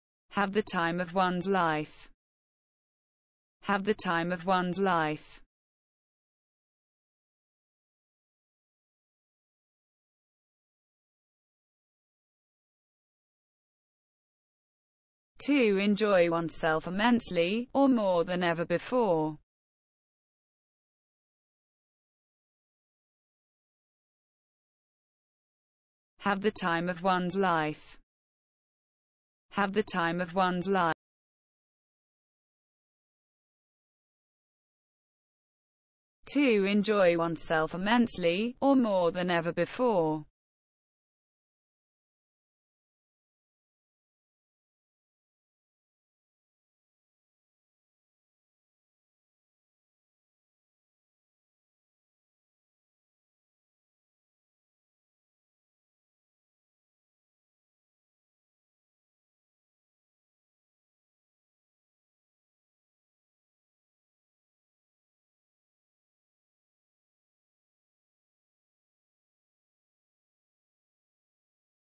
What does Have the time of one's life mean?
What does Have the time of one's life mean in English? Learn the meaning of the word Have the time of one's life! How to Pronounce Have the time of one's life. Expand your vocabulary, learn words.